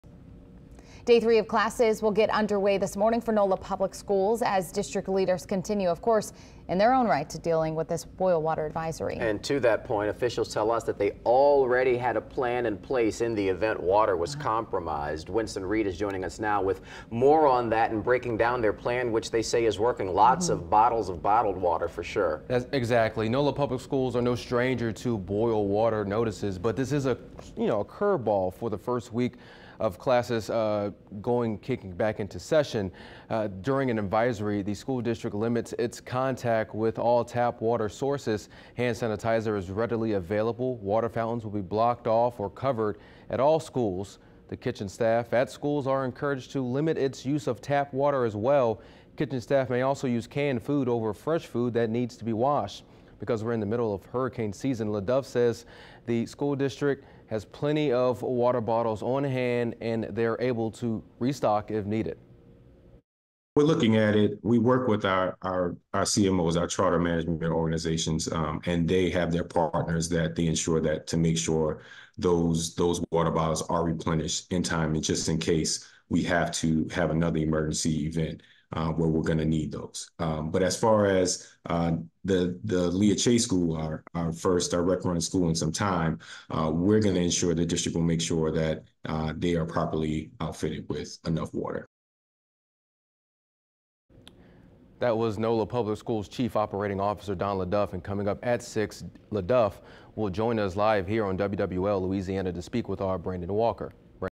Citywide boil water advisory impact on schools
Officials tell us they already had a plan in place for the crisis.